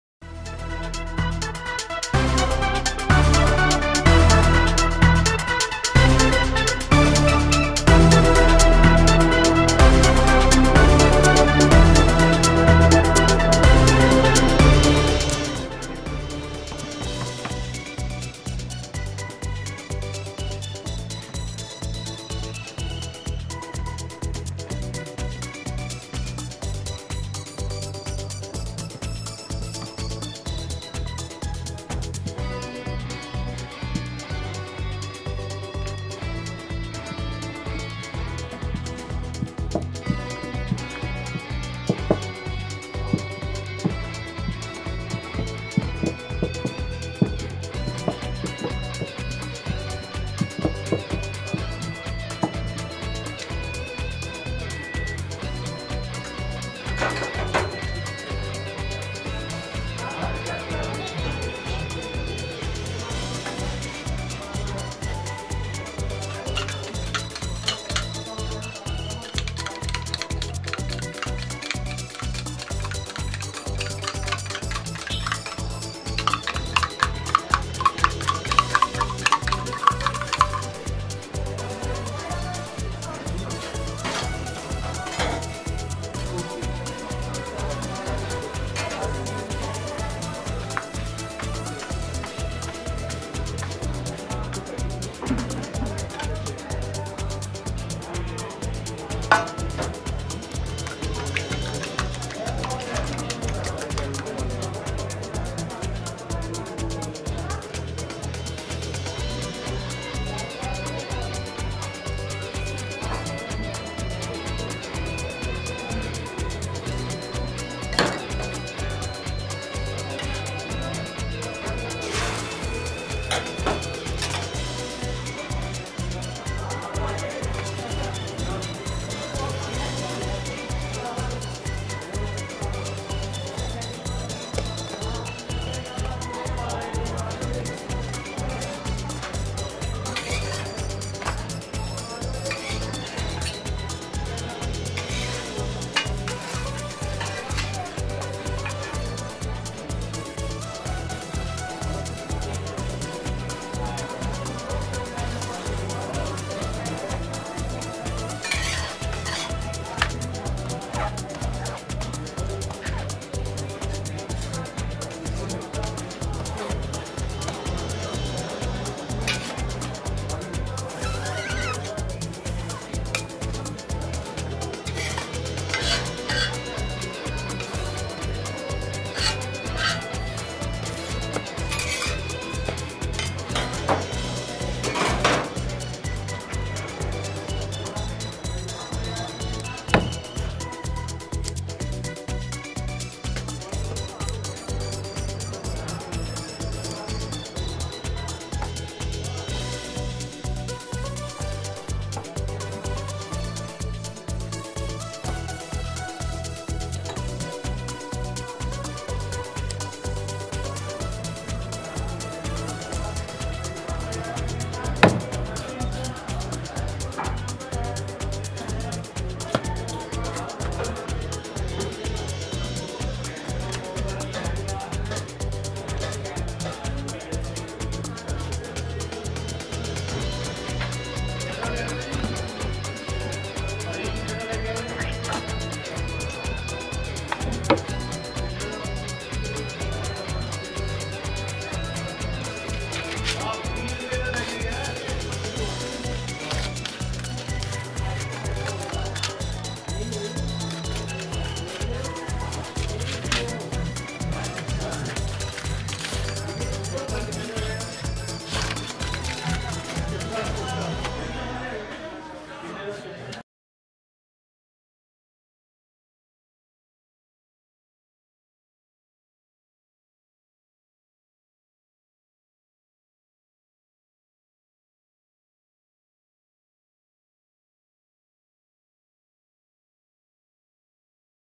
Restaurant Style Creamy Chicken Tikka Roll | Amazing Indian Street Food | Stuffed Chicken Wrap
Hello Everyone!

if you're a lover of chicken tikka rolls, this is the perfect spot to get chicken tikka rolls in Chandigarh. Everyone craves something light and easy to eat. So we would like to suggest you "ROLL ON FIRE" who makes the creamiest Chicken Malai Tikka Rolls. This restaurant focuses on serving food that is both unique and satisfying. They also offers rolls, burgers, sandwiches, and shakes. You must try the dishes at this restaurant if you visit Chandigarh.

By like and sharing this video, you can show your support for high-quality street food.